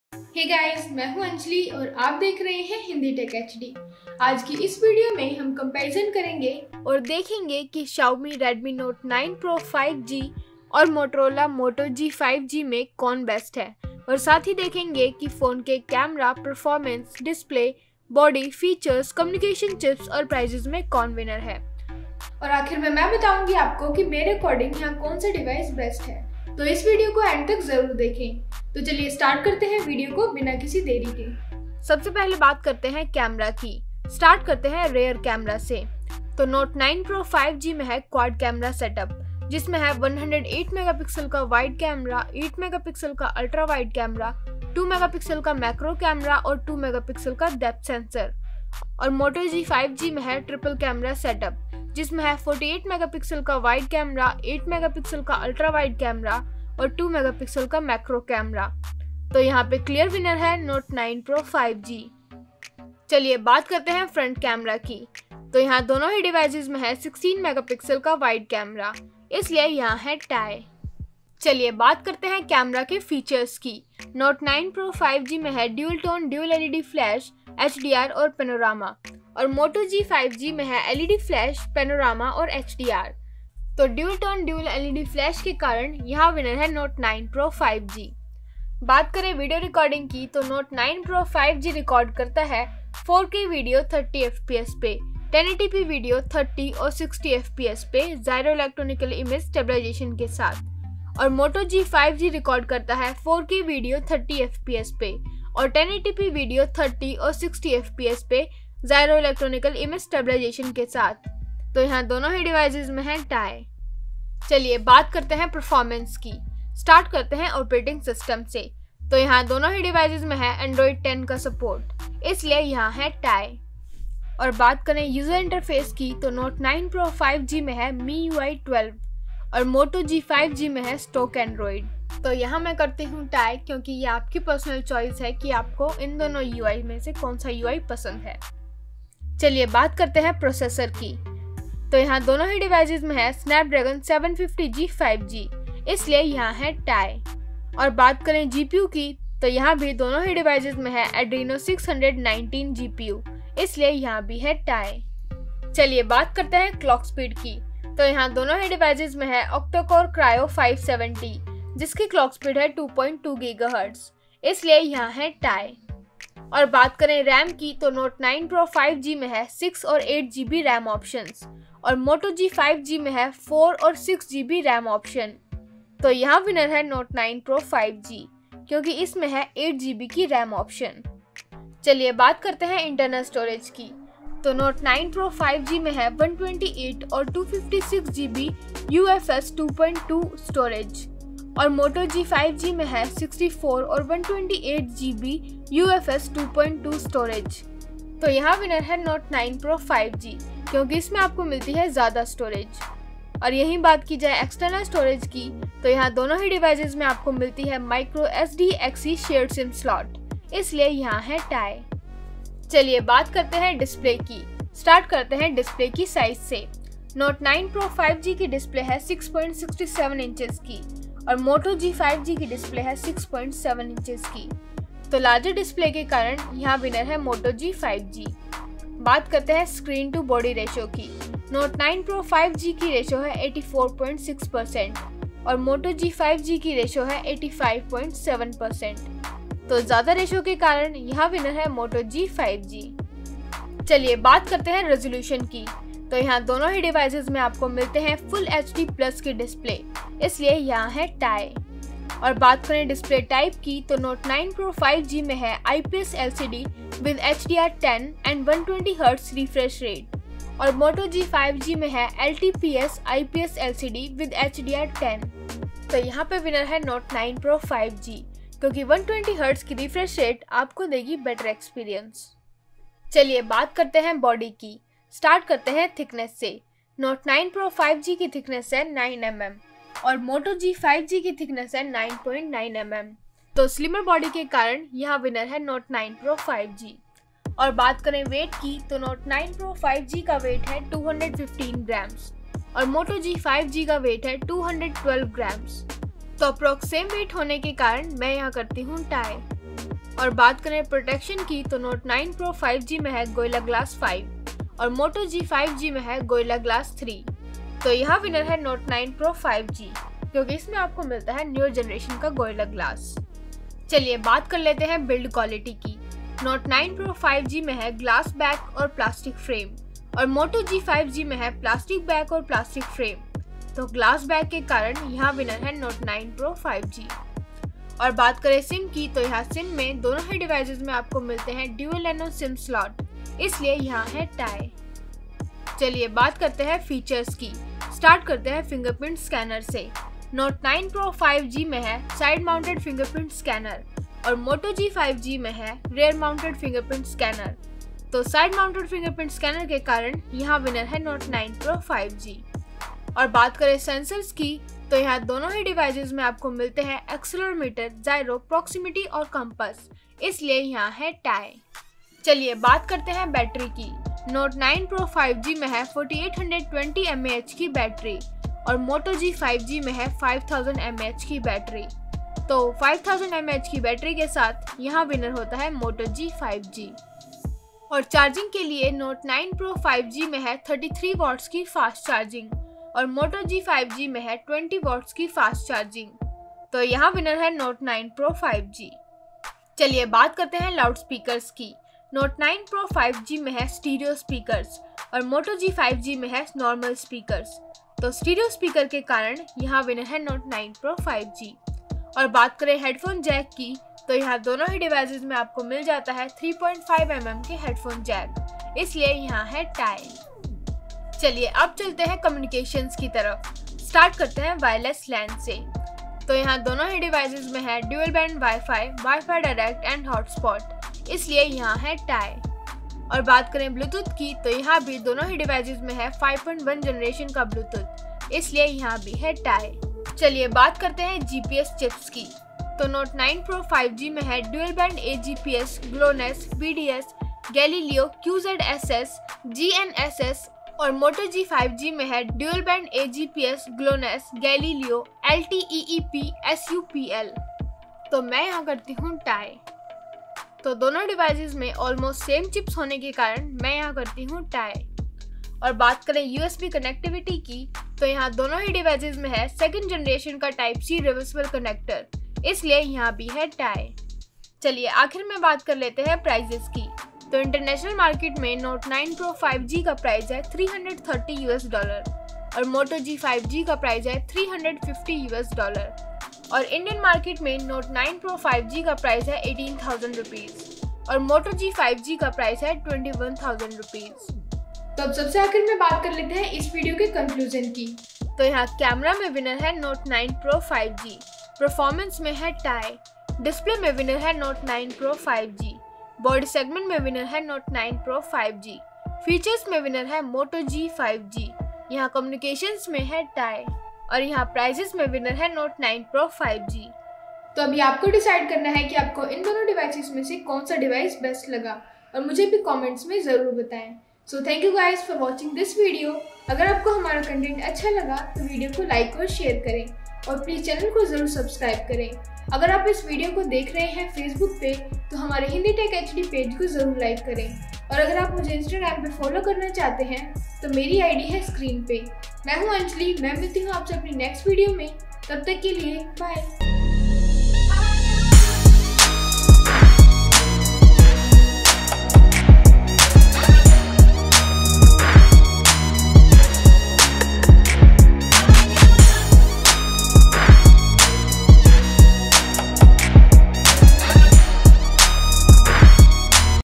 Xiaomi Redmi Note 9 Pro 5G vs Motorola moto G 5G | Which one is BEST BUY? | Full Comparison
In this video, we compare Xiaomi Redmi Note 9 Pro 5G and Motorola Moto G 5G phones. both the Smartphones come with Good specs lets see which one is the best.